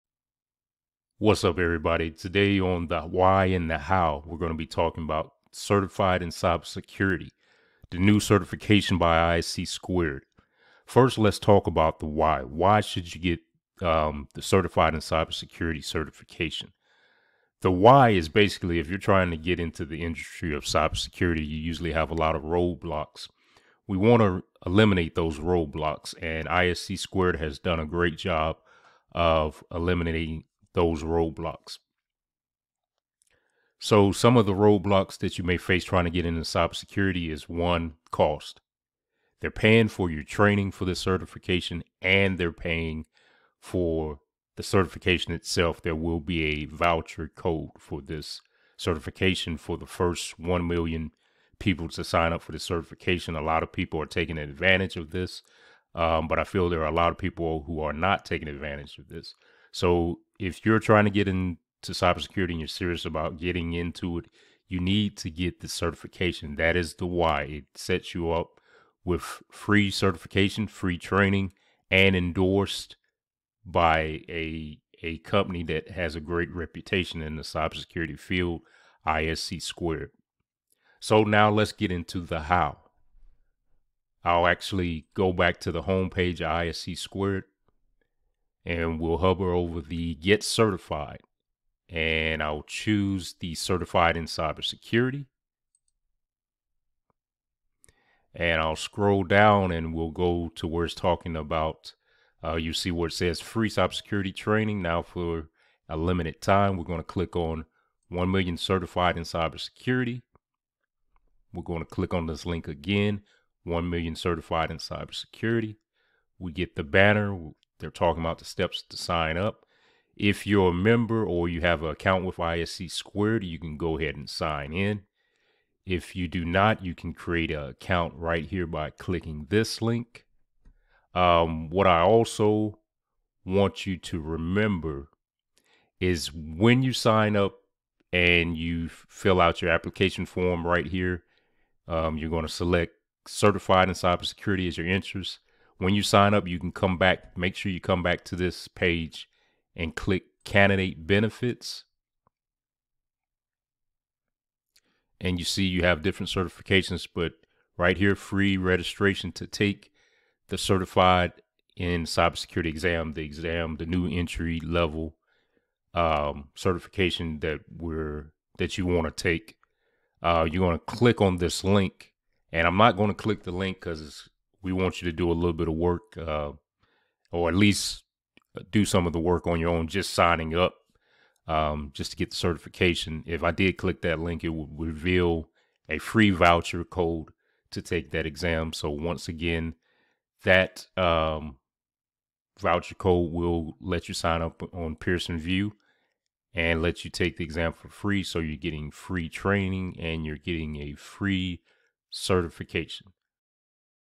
Becoming Certified in Cybersecurity Is A Great Career Move
You should get the new ISC2 Certified in Cybersecurity certification now!

Why:
-Free Training
-Free Certification
-ISC2 is highly respected in the cyber security industry.